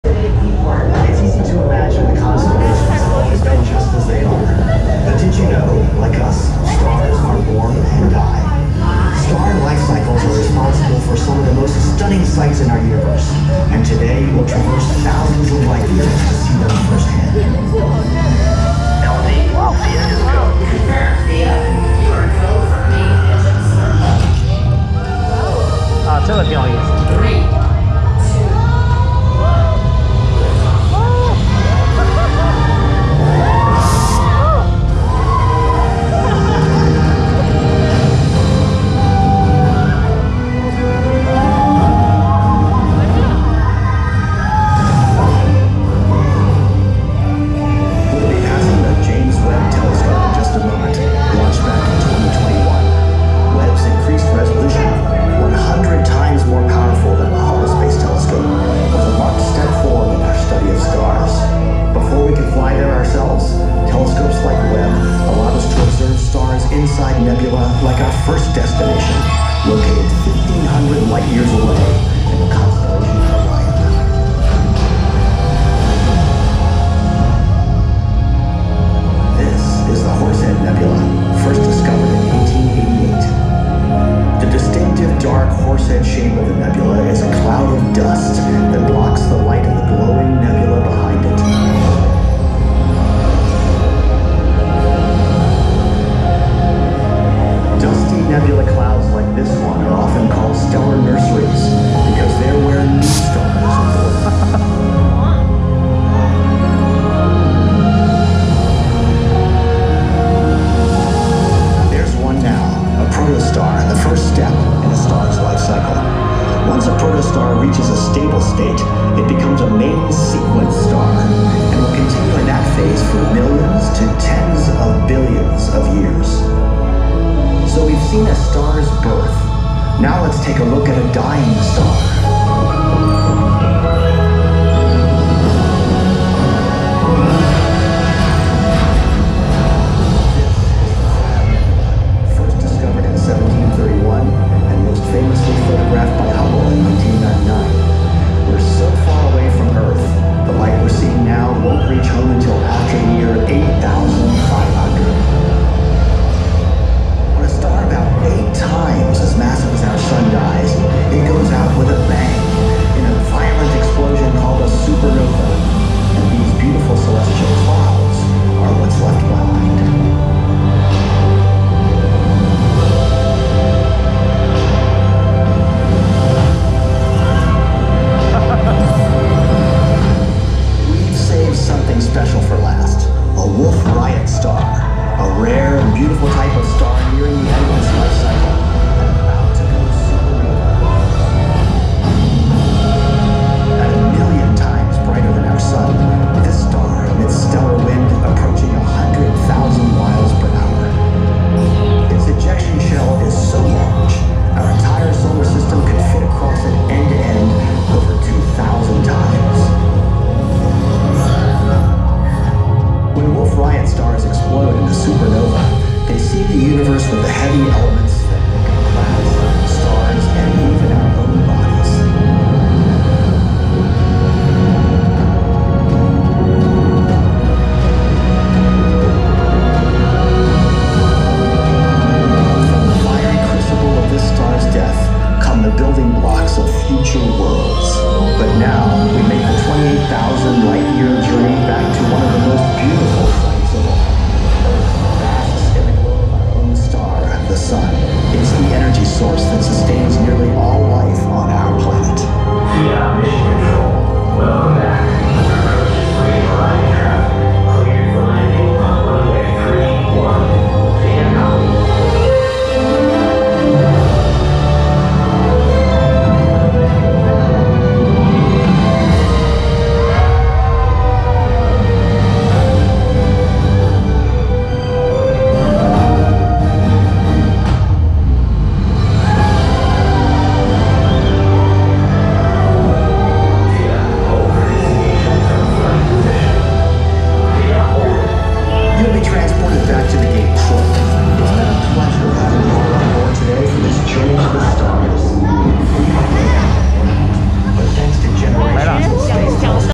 Spaceport Gateway Tour @ Kennedy Space Center
Tour in The Kennedy Space Center. The video was shot on January 16, 2023. Experience the future of a spaceship taking off from spaceport to travel the galaxy. Very interesting.
Due to the use of antique GoPro Hero5 Session shooting, the picture quality is relatively poor.